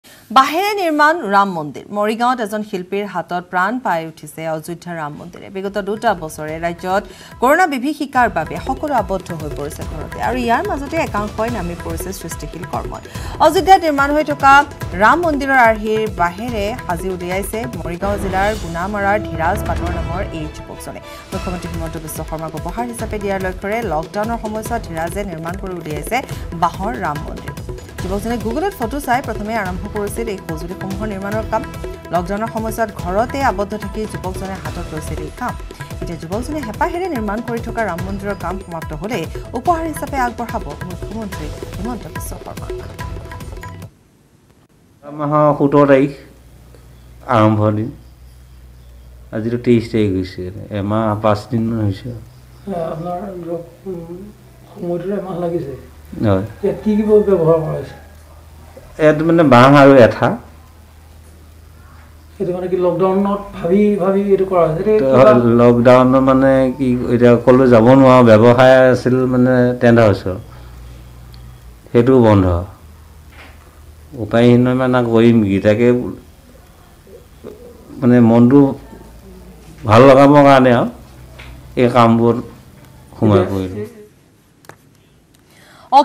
Morigaon News | মৰিগাঁৱত নিৰ্মাণ হৈছে বাঁহেৰে ৰাম মন্দিৰ
23 July, 2021: এগৰাকী শিল্পীয়ে বাঁহেৰে সাজি উলিয়াইছে ৰাম মন্দিৰ । লগতে, News18 Assam Northeastত চাওঁক আন কেতবোৰ গুৰুত্বপূৰ্ণ সংবাদ।